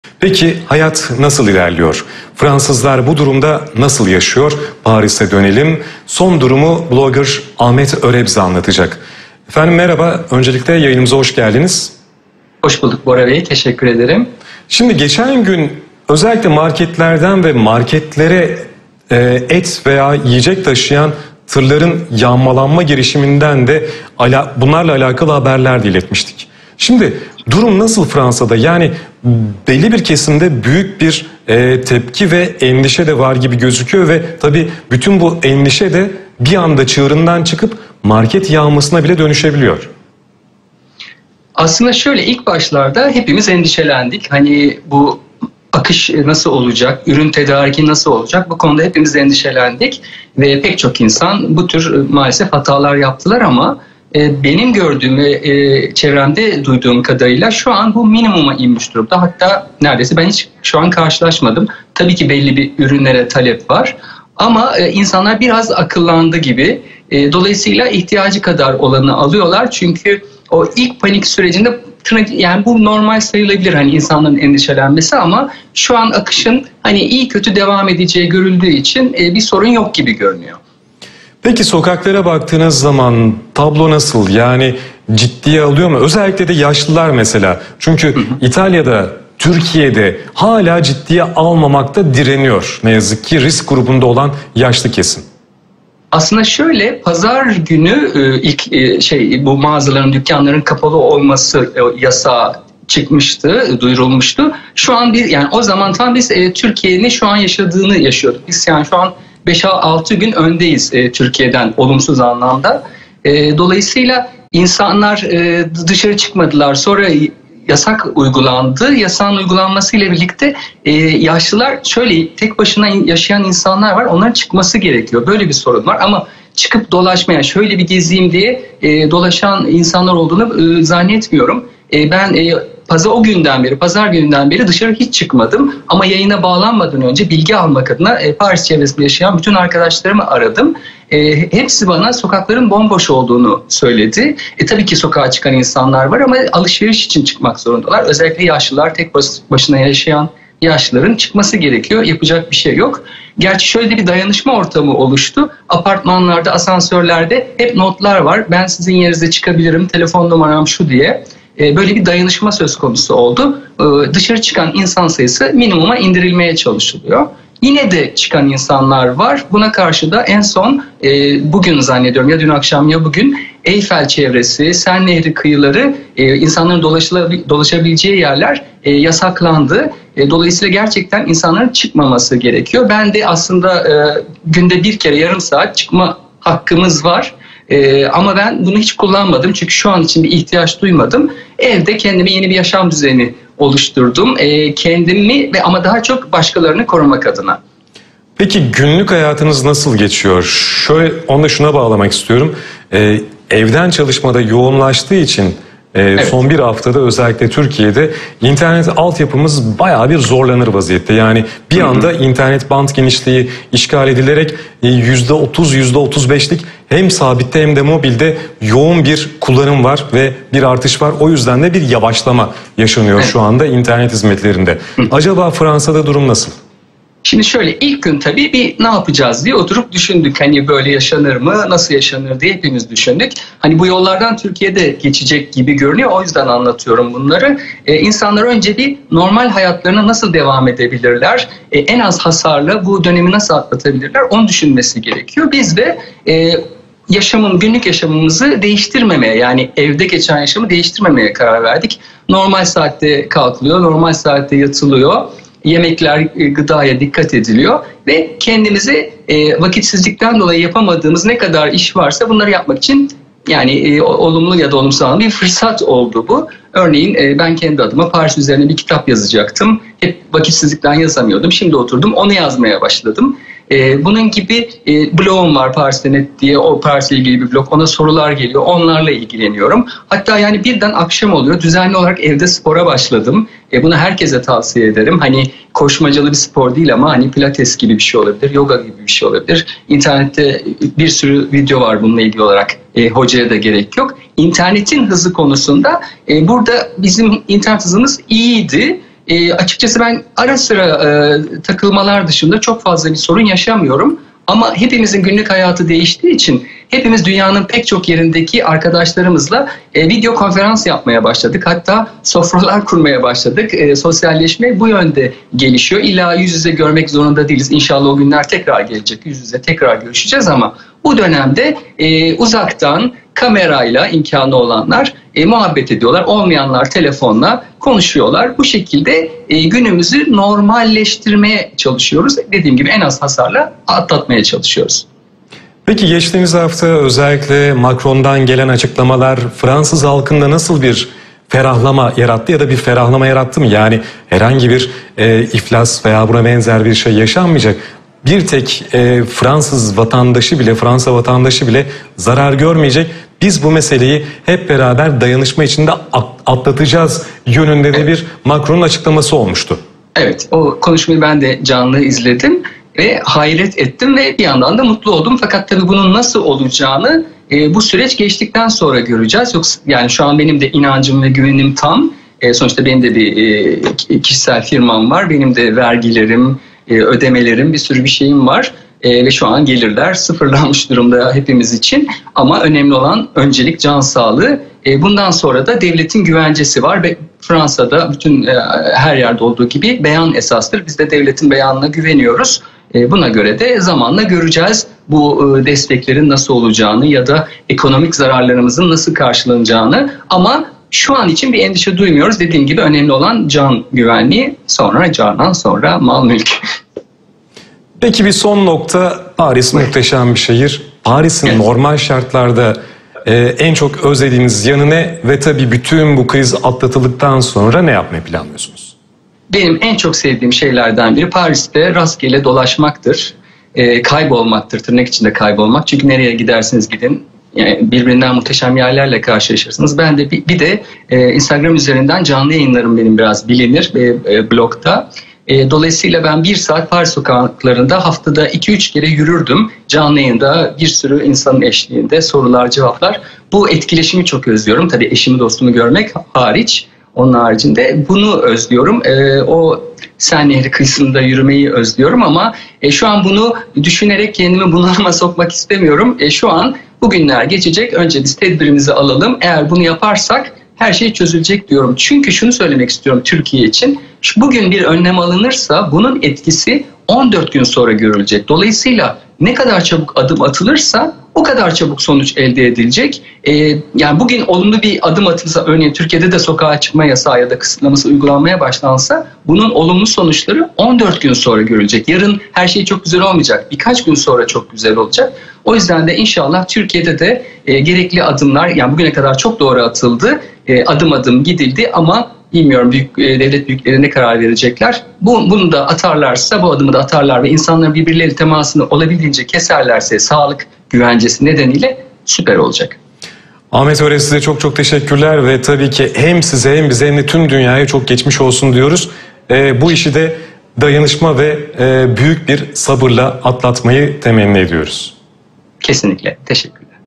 Haber Global TV Canlı Yayın Bağlantısı - Corona Günlerinde Paris
Fransa’da Covid-19 Coronavirus salgınını önlemek için ilan edilen sokağa çıkma yasağının, daha doğrusu sokağa çıkma kısıtlamasının uygulanmaya konmasının ardından Paris’te neler yaşandı? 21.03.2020 Cumartesi Haber Global Tv haber bültenine bağlanıp gerçekleştirdiğimiz canlı yayının videosunu (yayını kaçıranlar için) sizlerle paylaşmak istedim.

Daha sonraki olası canlı yayınların videolarını da görüntüleri elde edebilirsem paylaşacağım.

Kendinize çok iyi bakın, sağlıcakla kalın.

Ahmet ÖRE
Paris